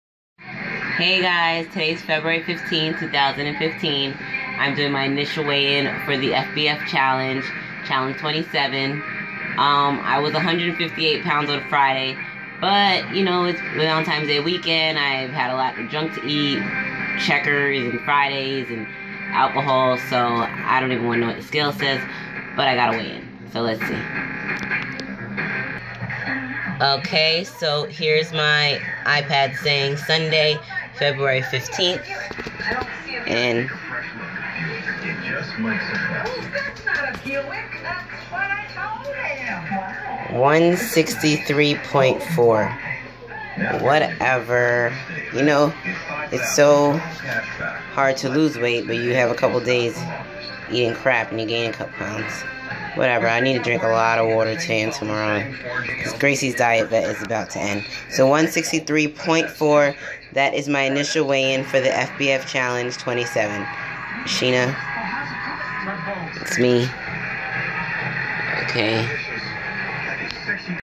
Initial weigh-in FBF Challenge 27 ! FACE THE SCALE!!!
Starting another FBF challenge 2/16/15! I hit top 5 last challenge, I will do better this challenge :)